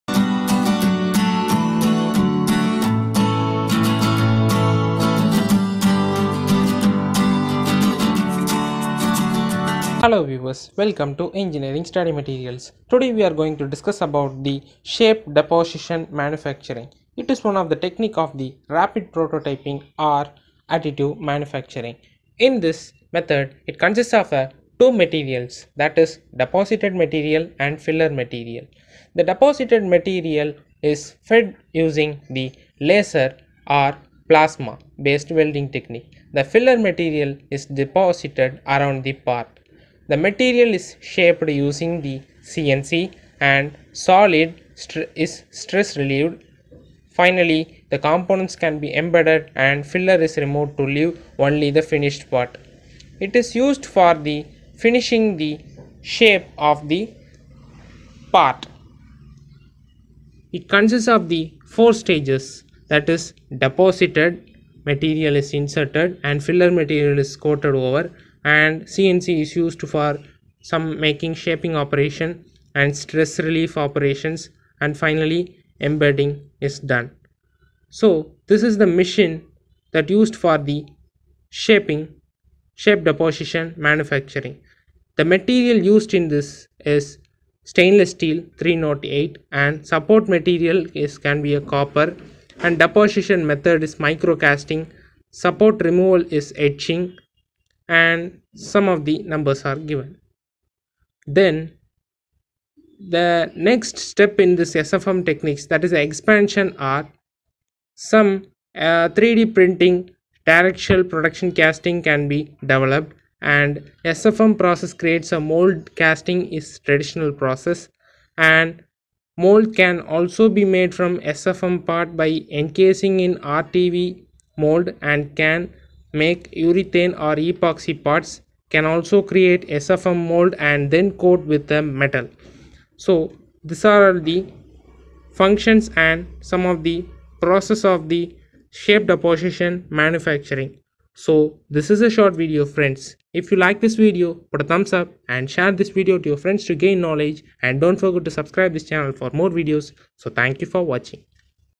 Shape Deposition Manufacturing| 3D printing techniques|Rapid Prototyping|ENGINEERING STUDY MATERIALS
SDM is a solid freeform fabrication process which means it is built from start to finish rather than by removing excess materials from a given object.  It does this by layering support material and the desired finished material.

    These graphics show the blue support material and the yellow final products.

Support material is laid down as a base for the final material.

Final material is laid on top of the support material.

CNC mills the part to the desired shape.

Surface is washed clean and prepared for the next layer of support and final material.

This process is repeated until the final layer is reached

Support material is removed.

How is design information transmitted to the manufacturing process?

    Part information is drawn in a 3D CAD model.  Then, the part is broken into thin two dimensional layers that are sent to the SDM machine.  This tells the machine where to place the support or part material and when a new layer is necessary.

What materials does SDM use?

    SDM can be used with any castable material especially plastics, metals, resins, polymers and ceramics.  When using ceramics, a mold is made of the support material and then the desired material is cast inside of it.
Mold  is made from mold material (wax) and support material.

Support material is removed leaving the wax mold.

The desired ceramic material is cast inside.

The mold is removed.

Any excess material is removed.